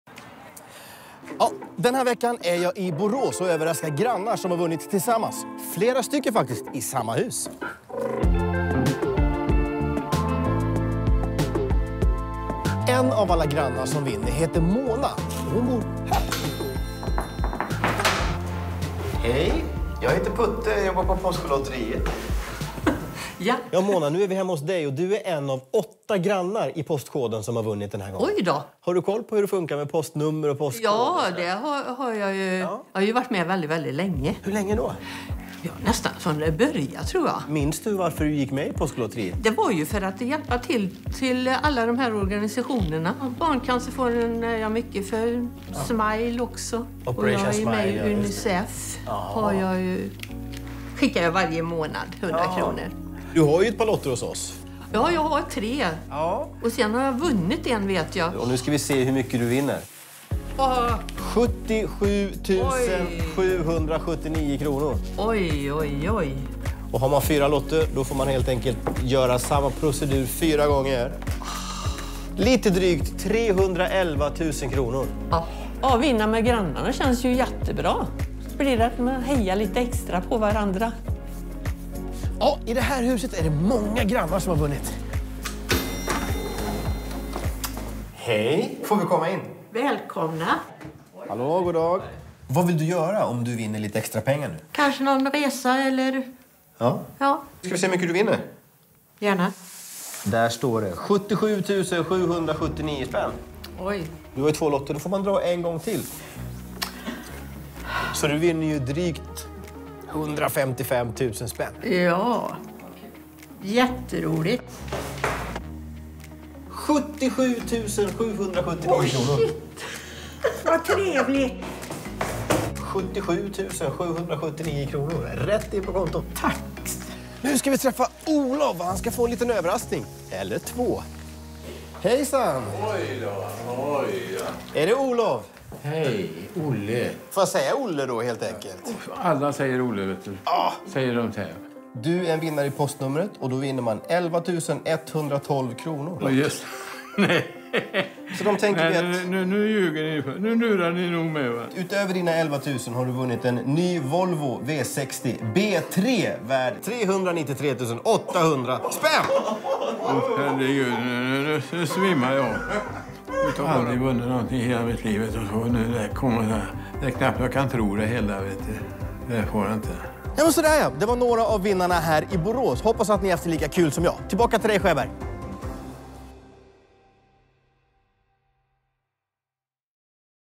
"Att vinna med grannarna känns ju jättebra!" - Postkodlotteriet
När Mona vann stort i Postkodlotteriet så kände hon glädje över att få dela vinsten med sina grannar. Hur skulle du reagera? Har du också fina grannar som du skulle vilja vinna tillsammans med?